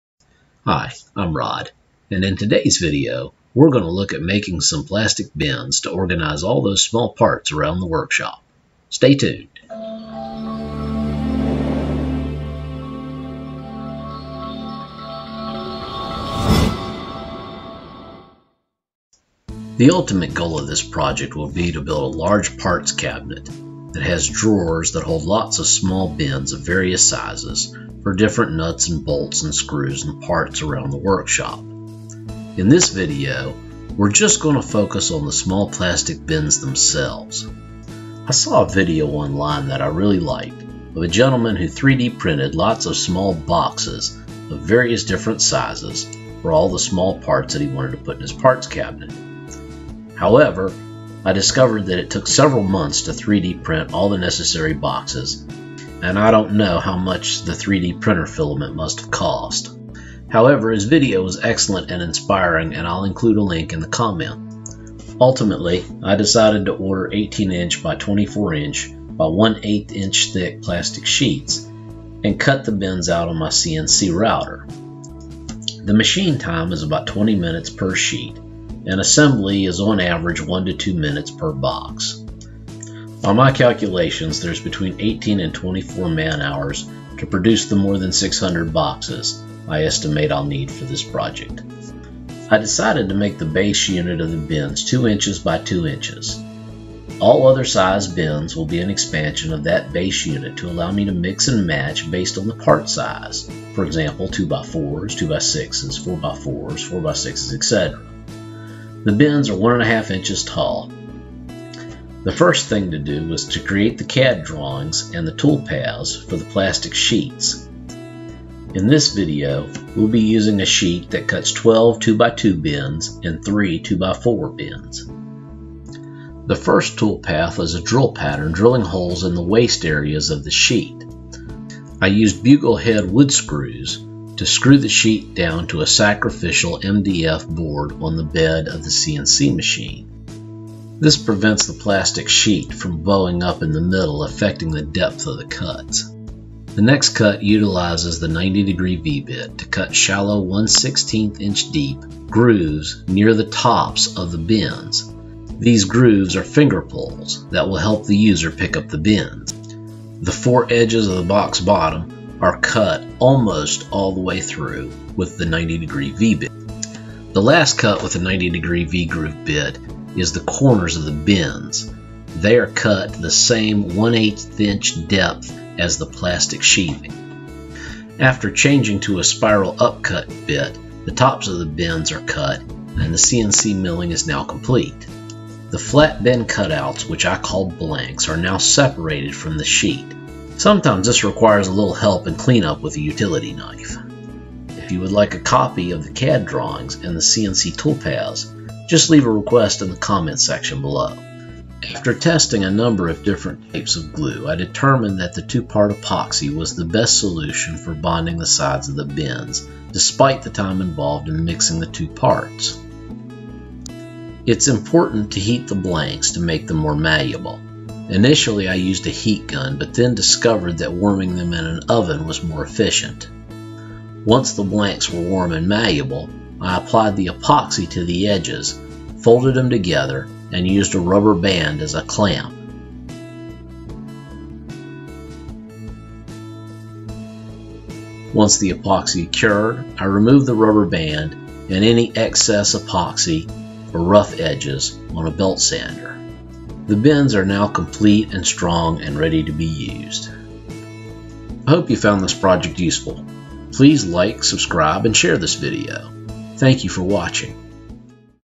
Plastic Parts Bins
Instructional project on how to efficiently make small boxes from plastic sheets utilizing a CNC router.